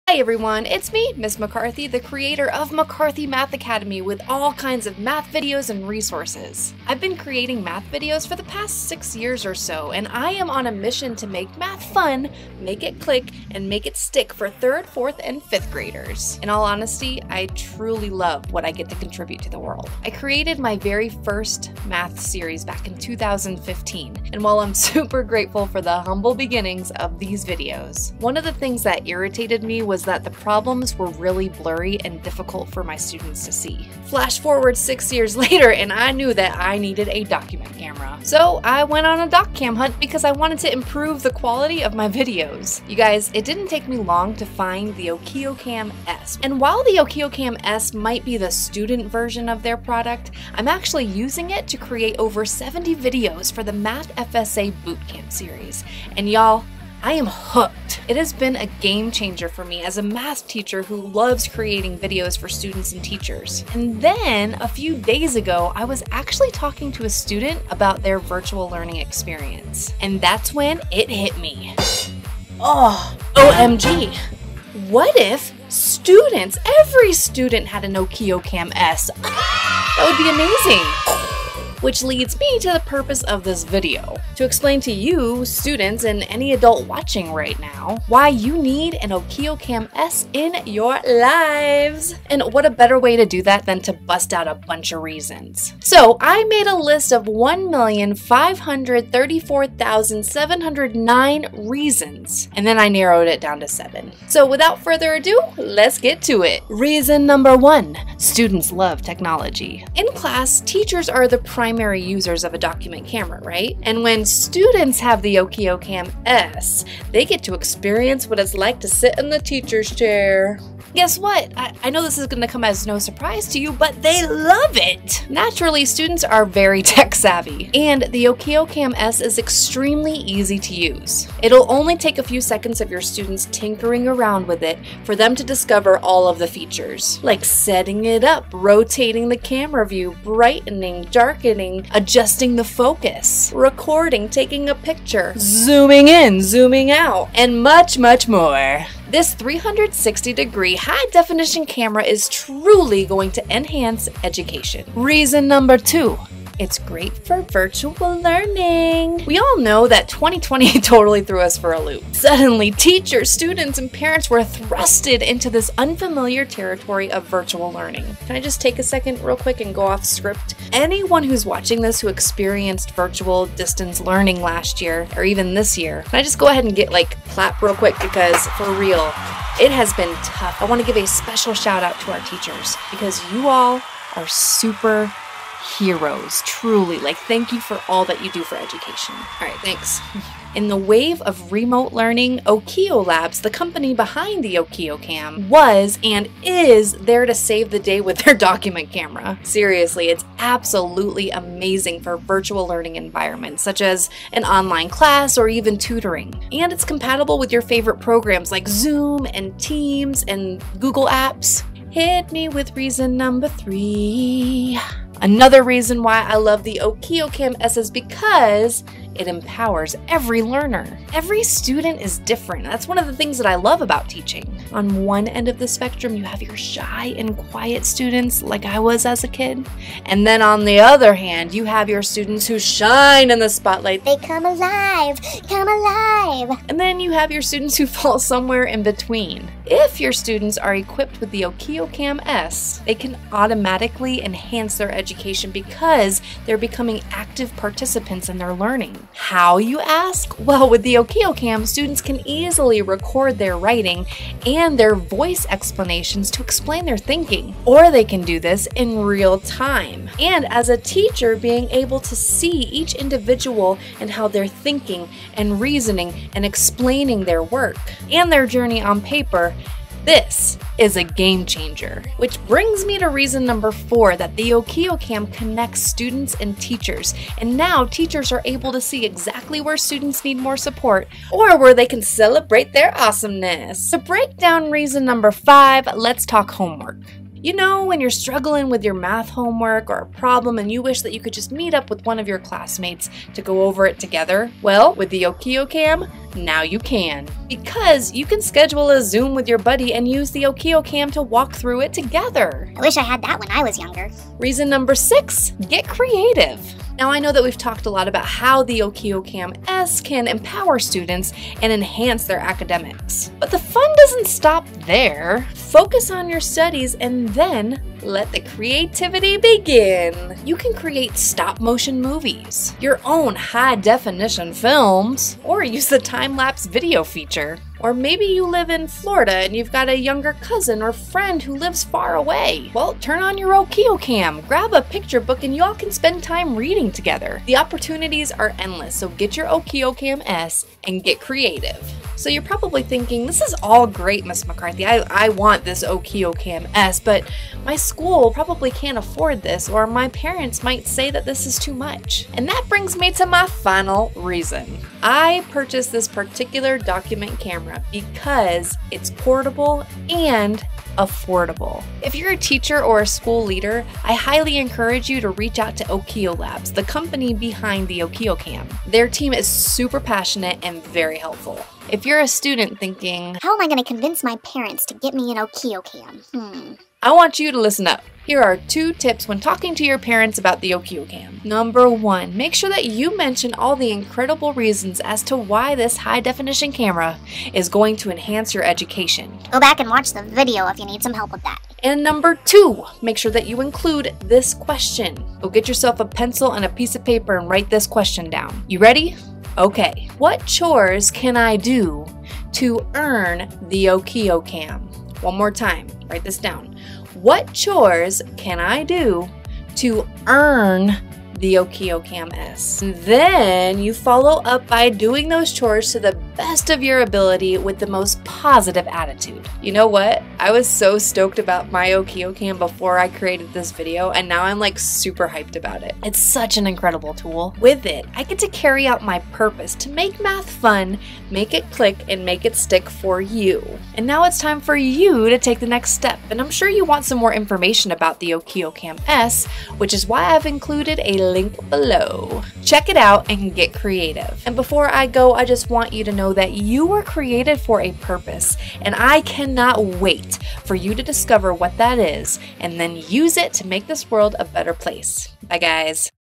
7 Reasons Why STUDENTS Need an OKIOCAM S in their LIVES!
I purchased the OKIOCAM S to film the Math FSA Bootcamp Series, and let me tell you. IT IS A GAME-CHANGER!

My first thought was that so many teachers could find incredible in the Okiocam. But then, it HIT me! You know who could really benefit from this powerful and portable document camera? STUDENTS!

And that's exactly why I created video to break it down for you! Check it out, and grab your OKIOCAM today! There is a reason why they have so many GREAT REVIEWS on Amazon. I seriously LOVE mine!

Okiocam S